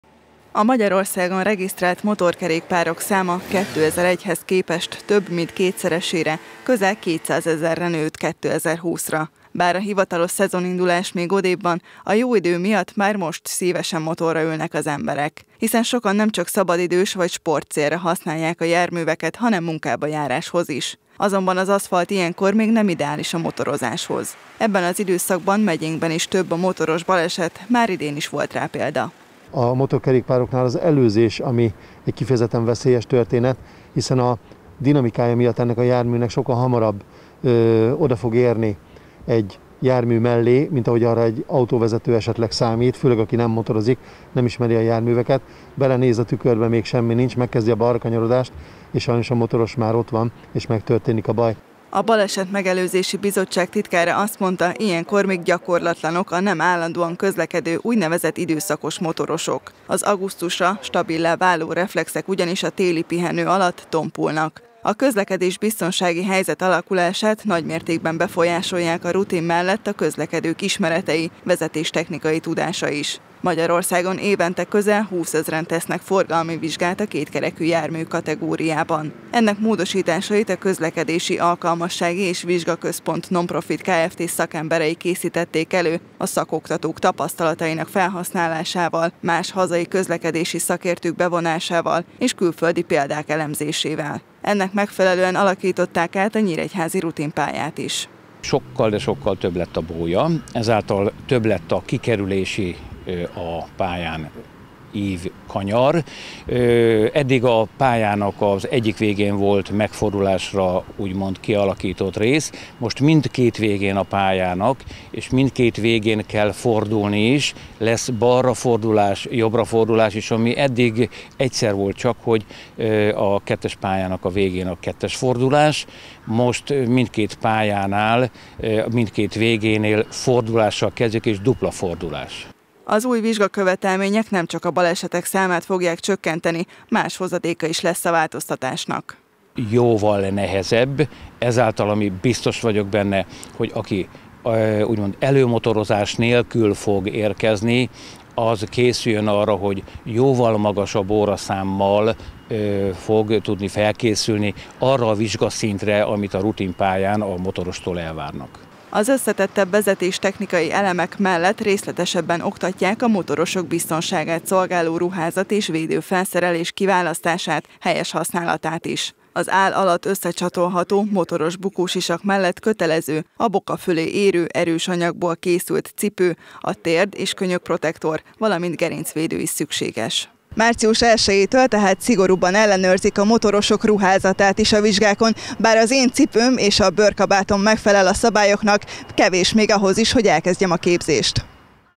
Megújult vizsgakövetelmények
Megújított tantervi és vizsgakövetelményeket teljesítve szerezhetnek jogosítványt a magyar motorosok 2021. március elsejétől. Az Innovációs és Technológiai Minisztérium alapos szakmai előkészítő munka és egyeztetési folyamat eredményeként korszerűsítette a képzést. A cél a balesetek számának csökkentése. Tavaly országosan közel 40 motoros volt halálos áldozata a közúti baleseteknek.